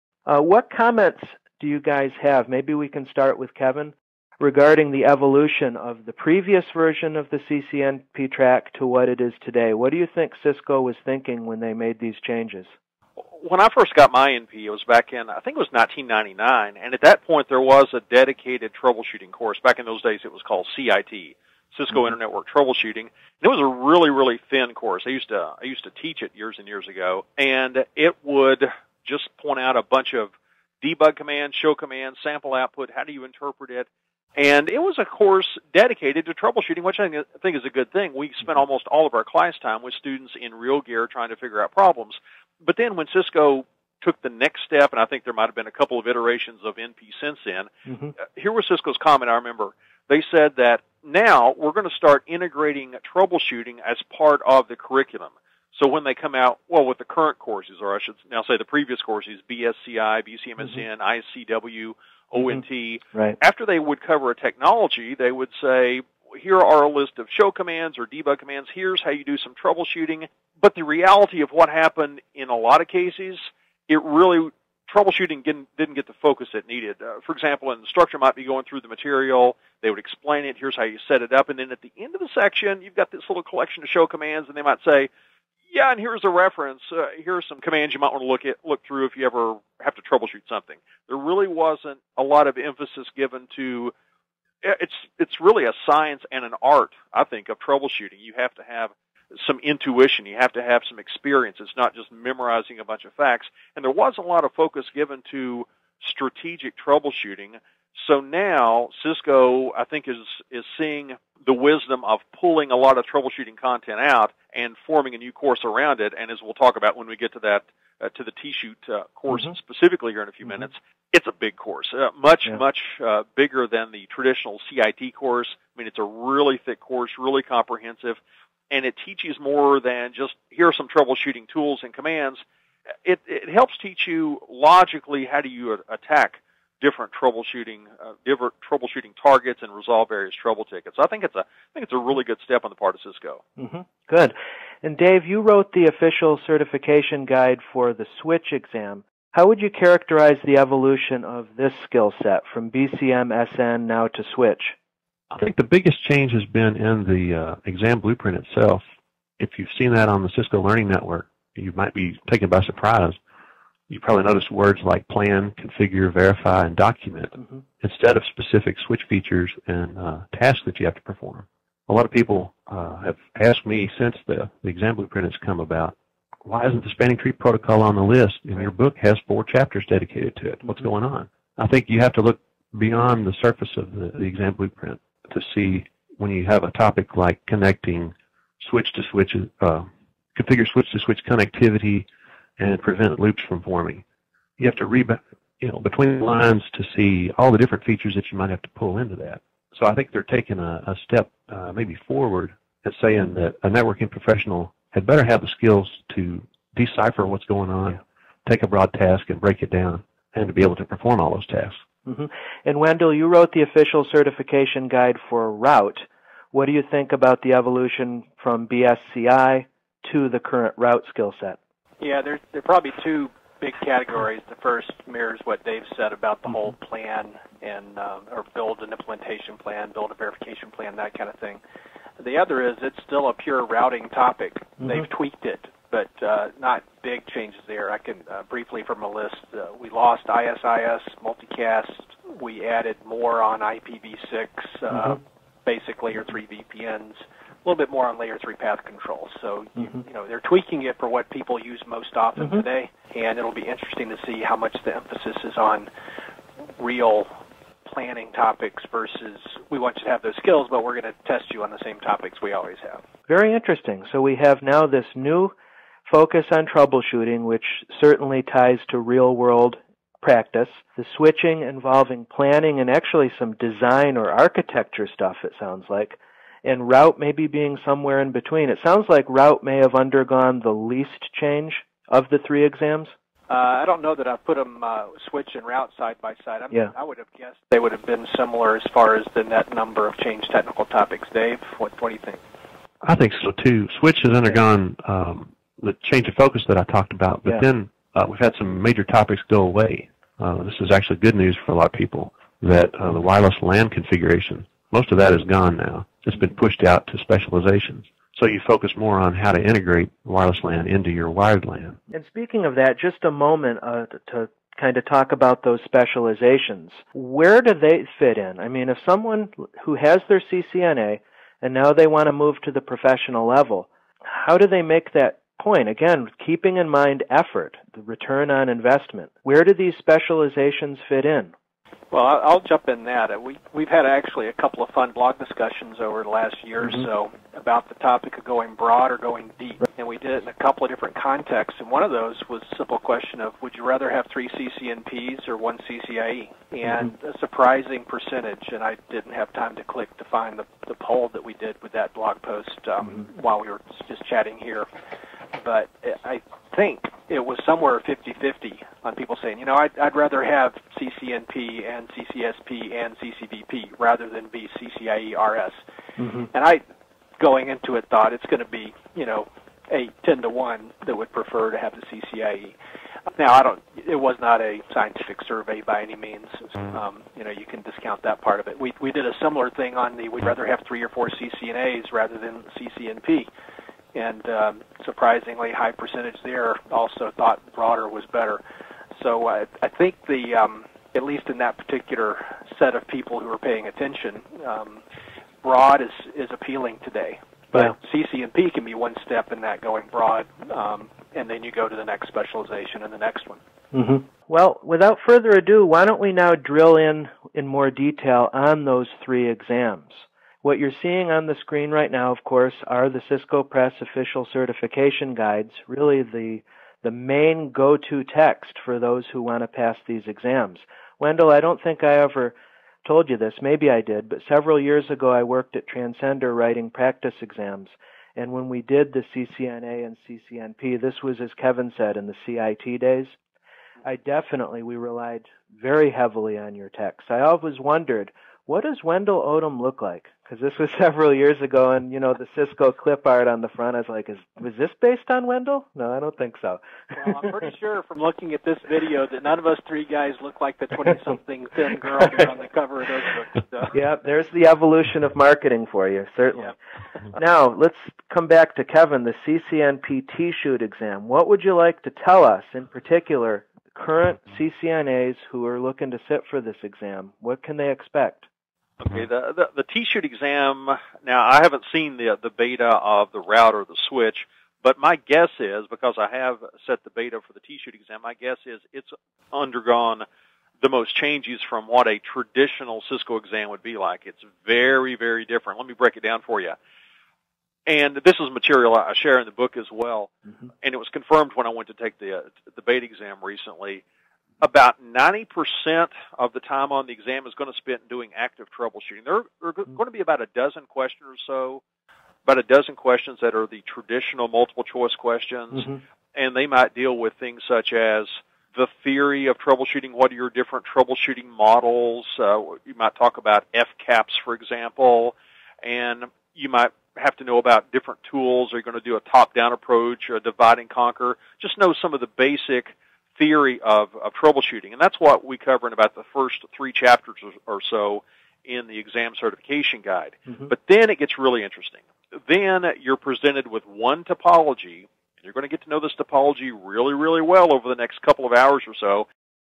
Cisco CCNP Roundtable Discussion: Part 2/4
Tim Warner leads an informative discussion on the new Cisco Certified Networking Professional (CCNP) track with three leading Cisco Press authors: Wendell Odom, David Hucaby, and Kevin Wallace -Part 2.